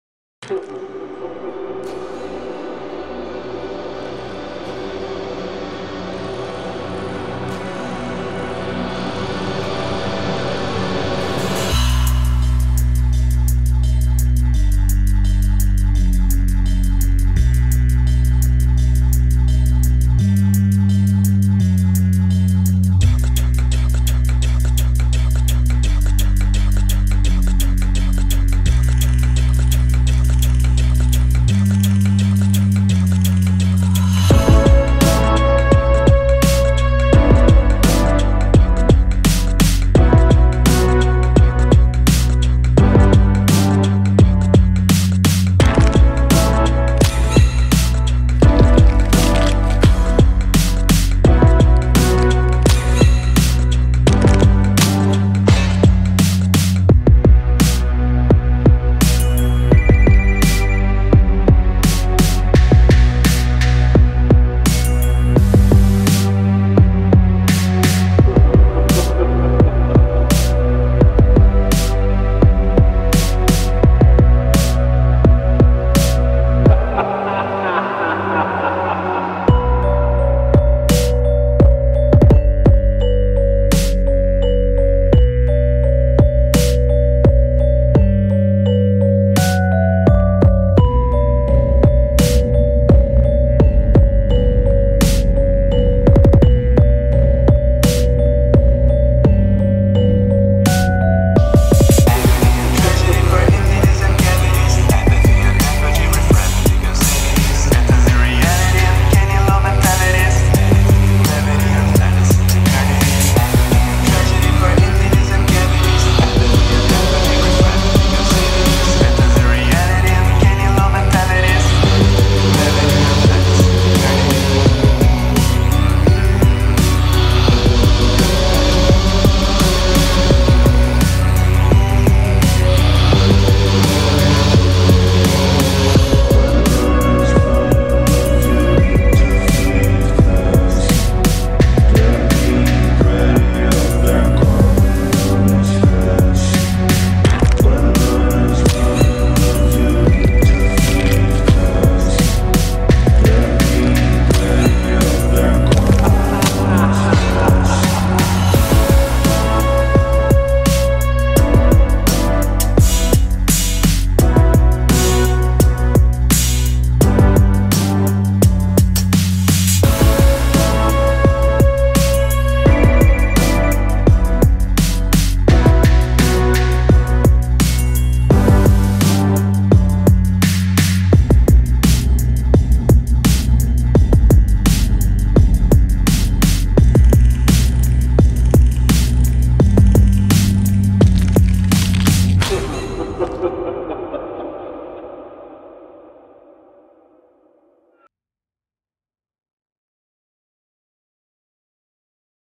Express Unofficial Single
Just a little thing i made with the sounds in express for fun
I really like express probably my favorite mod sound wise